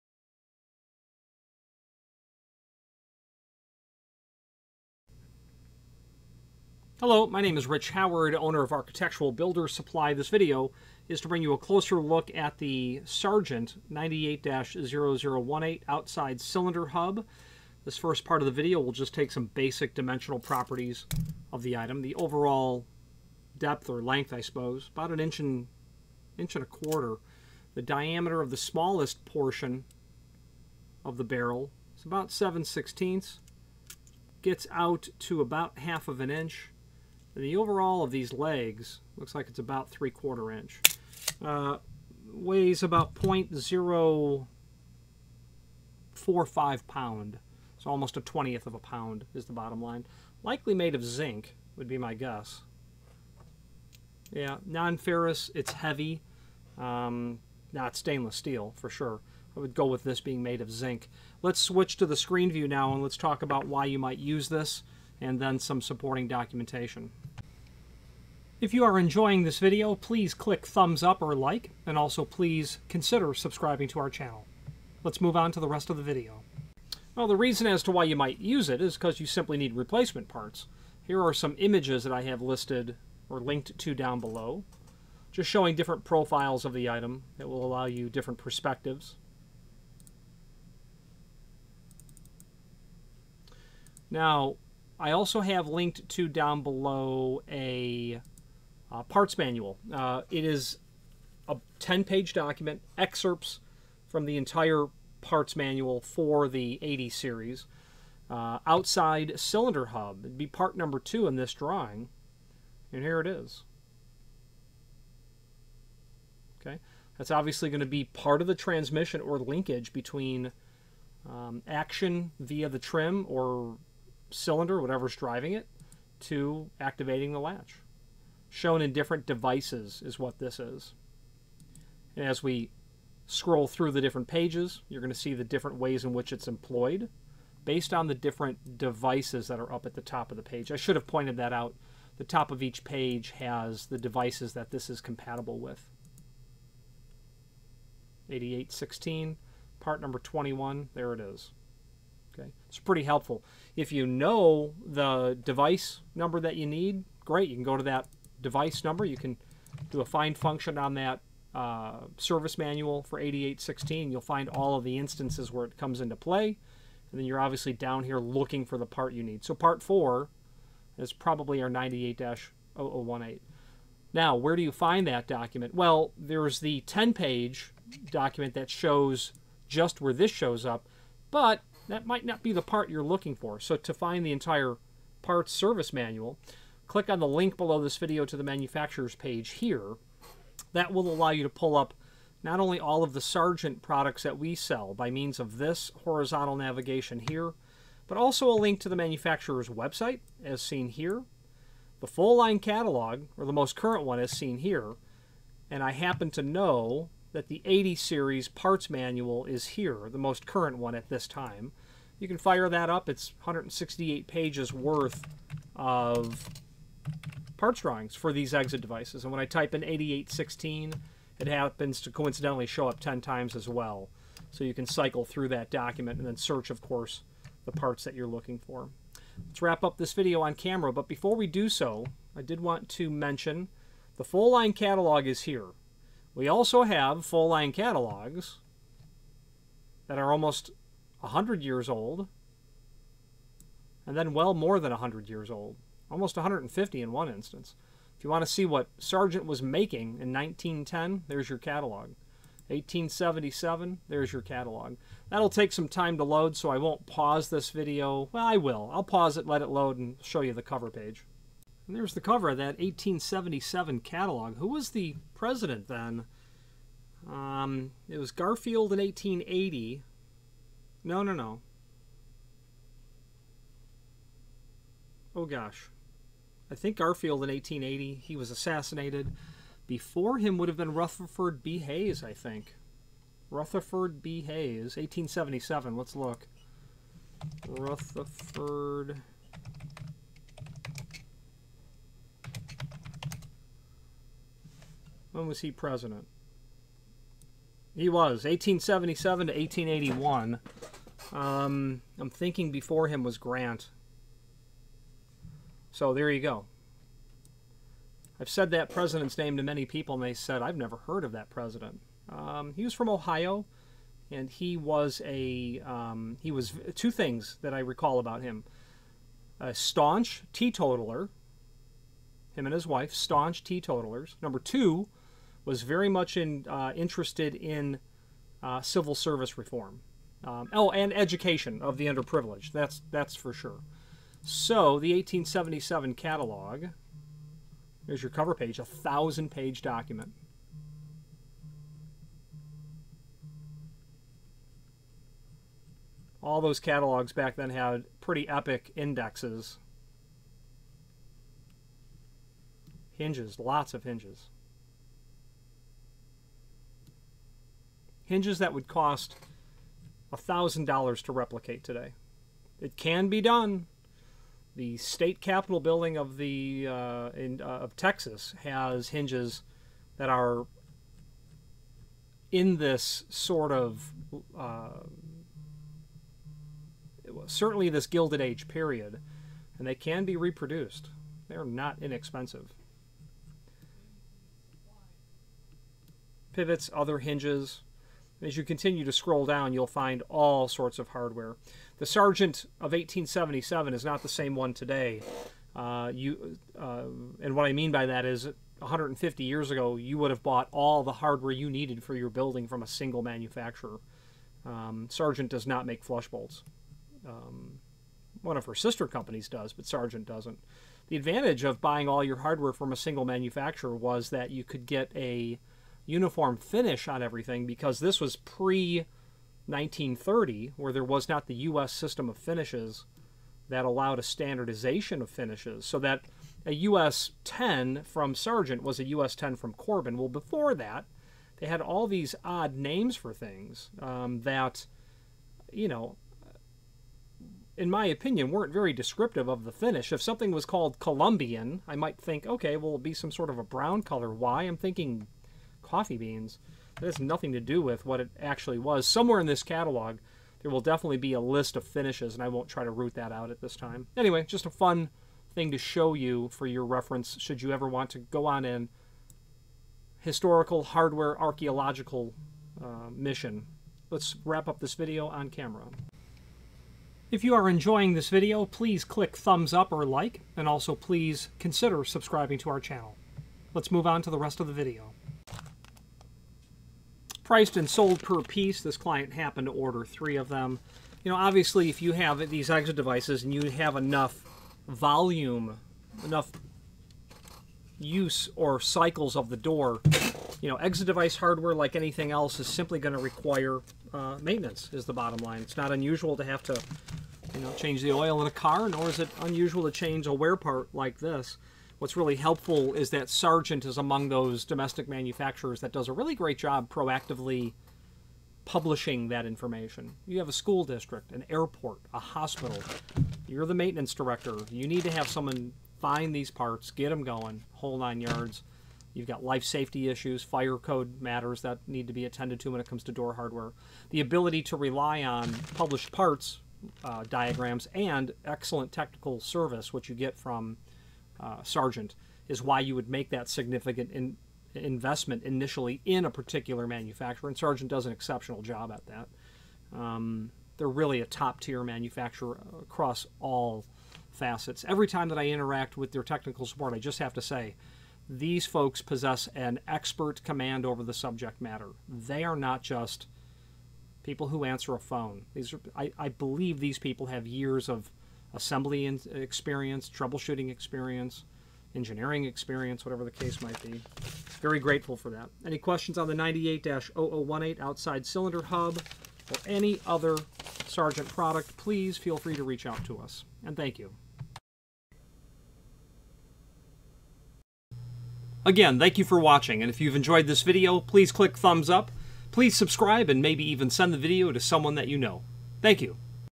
Sargent Co 98 0018 Outside Cylinder Hub
This video from absupply.net is to bring you a closer look at the Sargent Co  98 0018 Outside Cylinder Hub where we will visually review the item along with its dimensional properties and its intended uses.